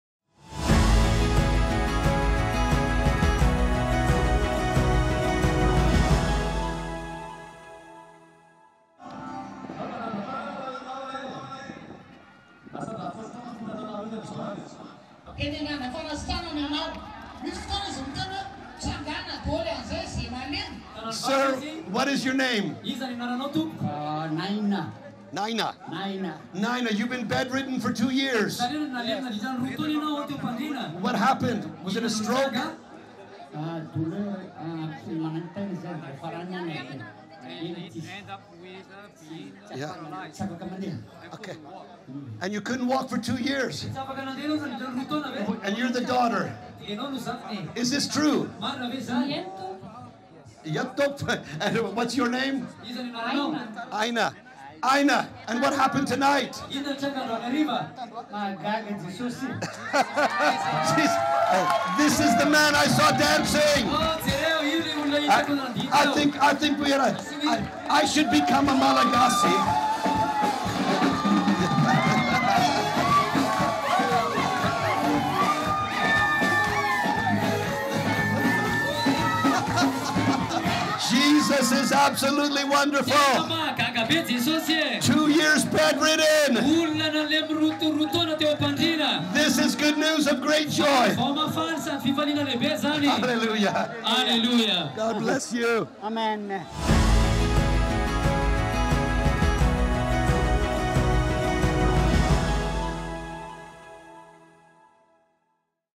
1 Minute Miracle - Mr. Naina's Legs Are Healed!
For two years, Mr. Naina was bedridden and could not walk from a stroke. He was brought to our festival in Fianarantsoa Madagascar, where Jesus completely healed his legs. He not only can walk, but dances too! If you enjoyed this video, share it with someone, and follow us for more miracle moments!